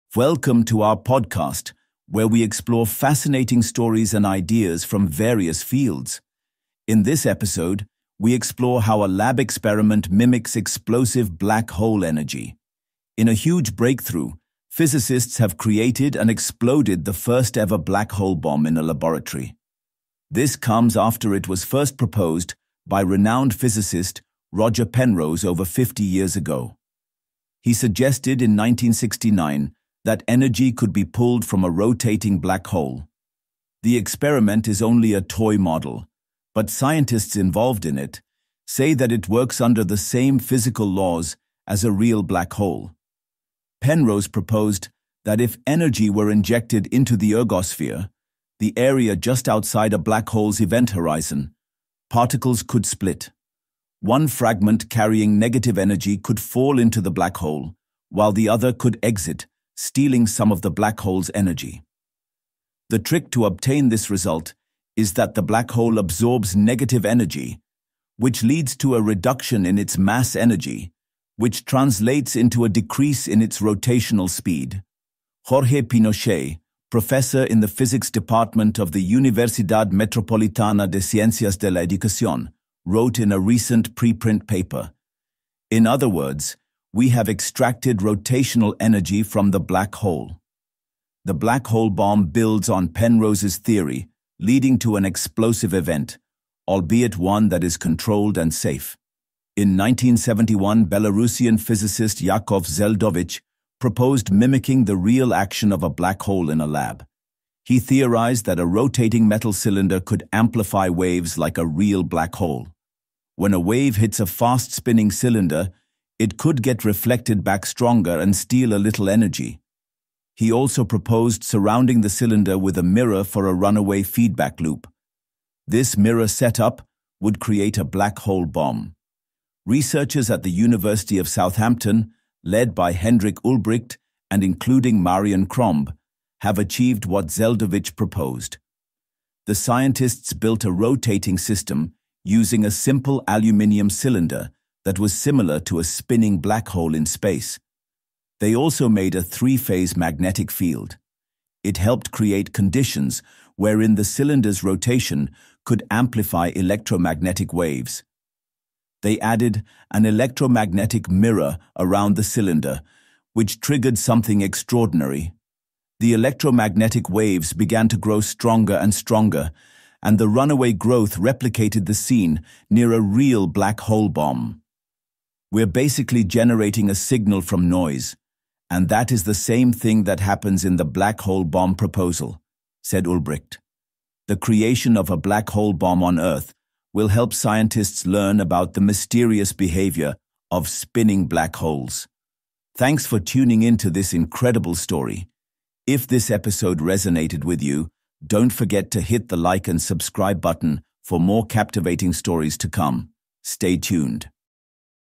Scientists Create First-Ever Black Hole Bomb in Lab | WION Podcast
In a groundbreaking achievement, physicists have successfully created and exploded the first-ever black hole bomb in a laboratory. Building on Roger Penrose's 1969 theory, the experiment demonstrates energy extraction from a rotating black hole. Though it's a "toy model," the experiment follows the same physical laws as real black holes. Researchers used a rotating aluminium cylinder surrounded by a mirror to replicate the black hole bomb phenomenon, offering valuable insights into the behavior of spinning black holes and energy extraction.